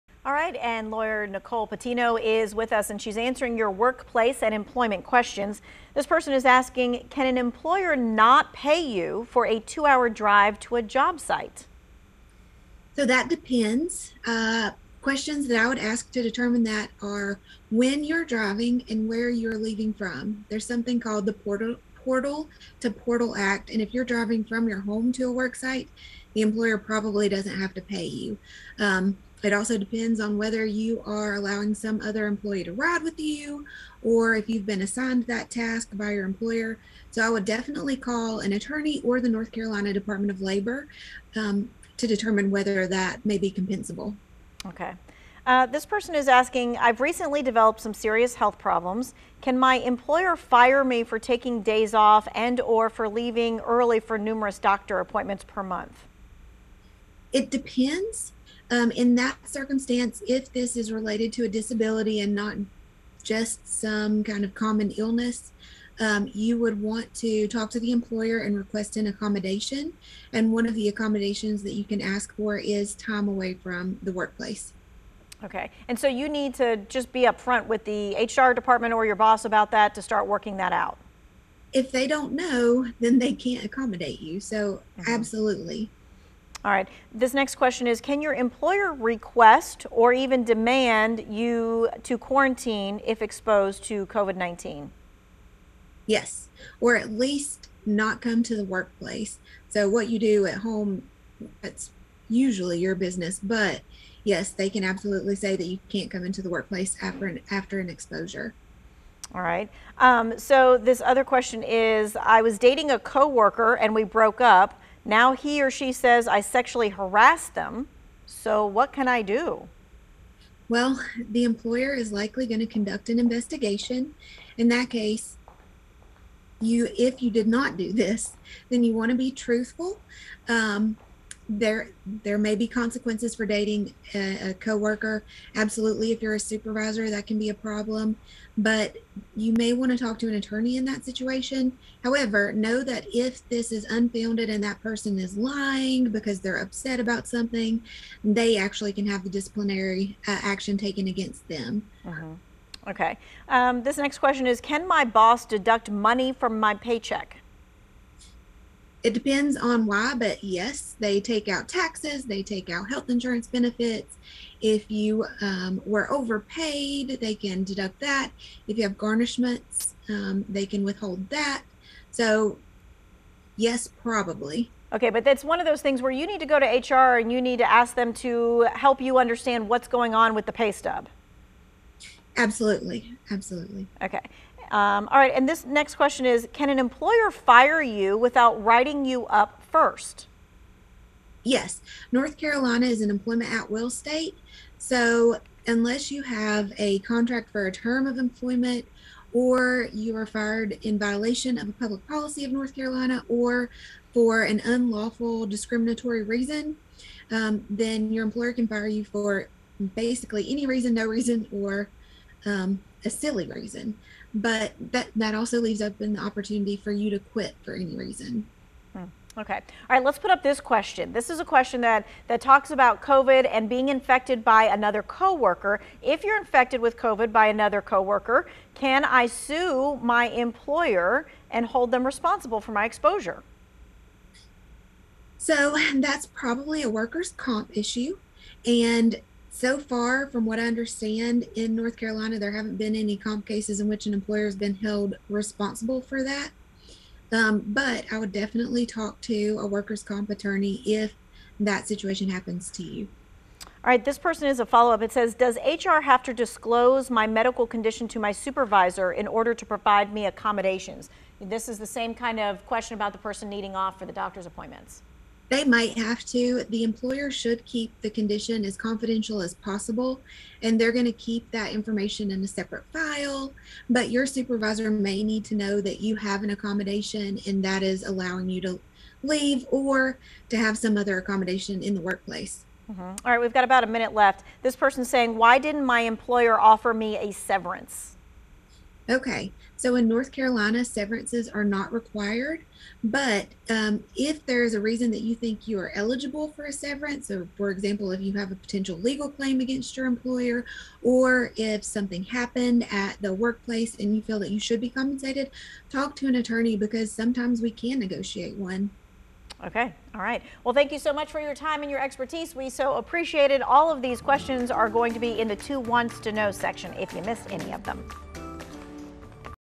Employment law attorney answers commonly asked questions | Part 2
Employee rights at work have come back to the forefront during the pandemic. From vaccine requirements to PTO for time missed, we know you have questions.

To answer them, labor and employment attorney Nicole Patino from the Law Offices of L. Nicole Patino joined 2 Wants to Know.

Employers in North Carolina can continue to require you to get vaccinated even though the OSHA mandate was struck down by the Supreme Court, according to Patino.

Patino said there are two exceptions. You can file for an exemption from the requirement if you have a sincerely-held religious belief that prevents you from taking the shot. You can also file for one if you have a disability.